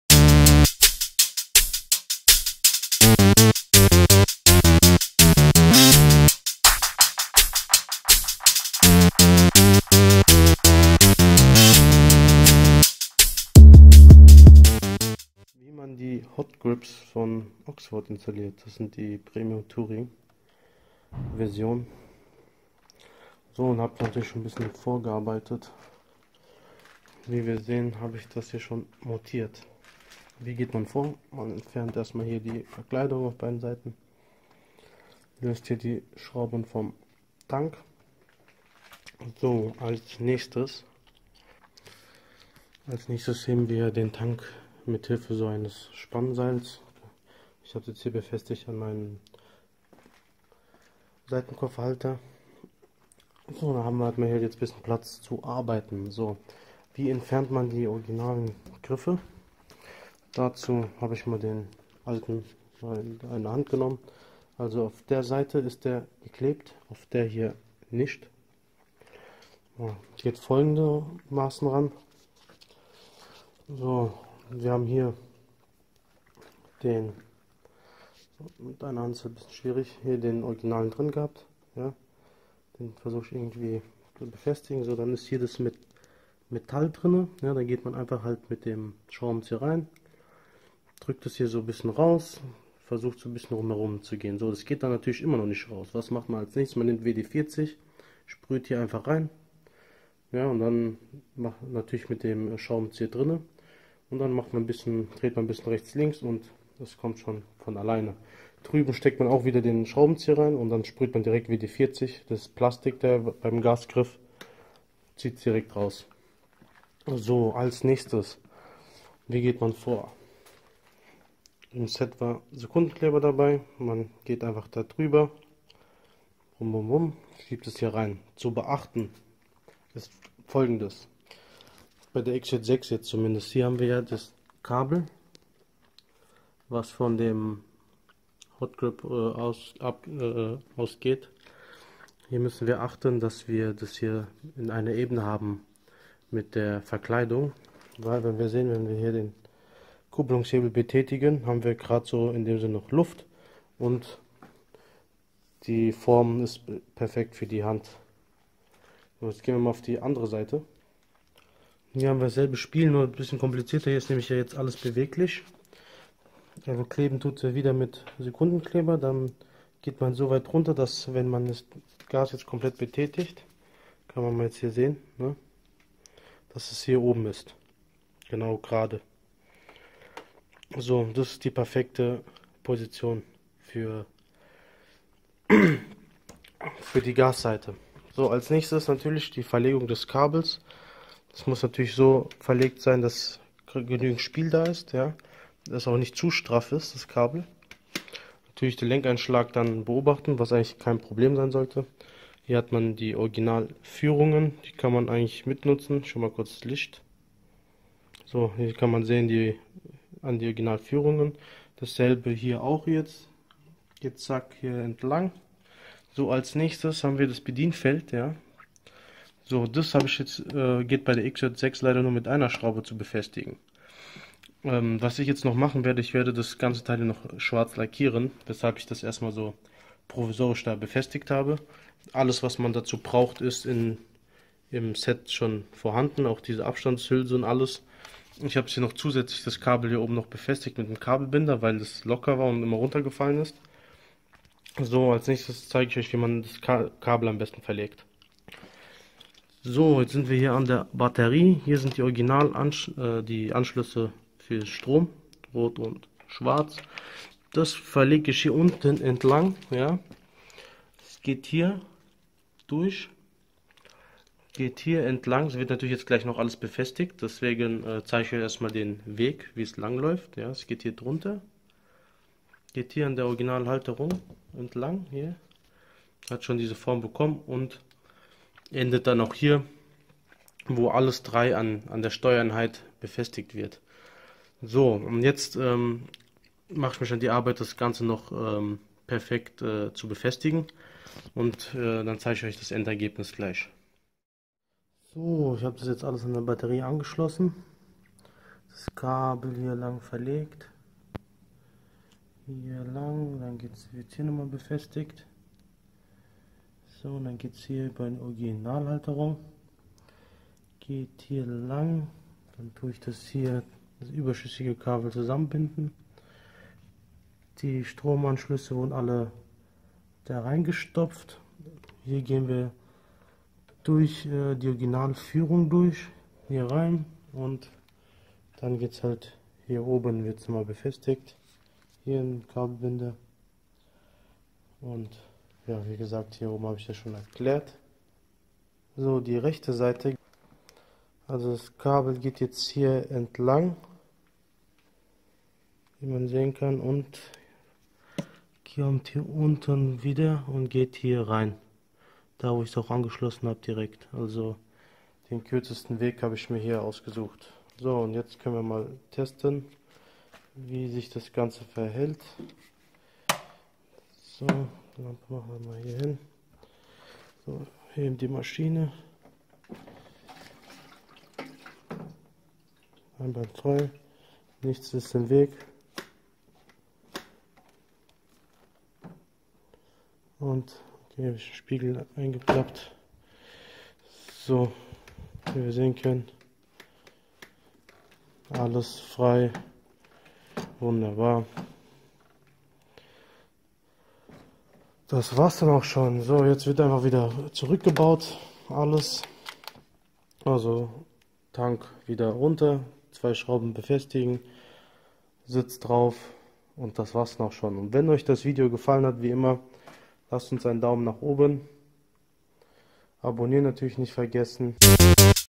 Einbau Griffheizung für die Yamaha XJ6 (Heated Grip Installation)
Hallo liebe Motorrad Freunde, in diesem Video Zeigen wir euch anhand der Yamaha XJ6 Diversion, wie ihr eine Griffheizung, in diesem Fall die von Oxford Hot Grips Premium Sport Touring, an eure Maschine montiert bekommt.

Hier noch die Links zu der Griffheizung:
Oxford Hot Grips Premium Touring

Oxford Hot Grips Premium Adventure

Oxford Hot Grips Premium Sport

Oxford Hot Grips Chopper

Oxford Hot Grips Scooter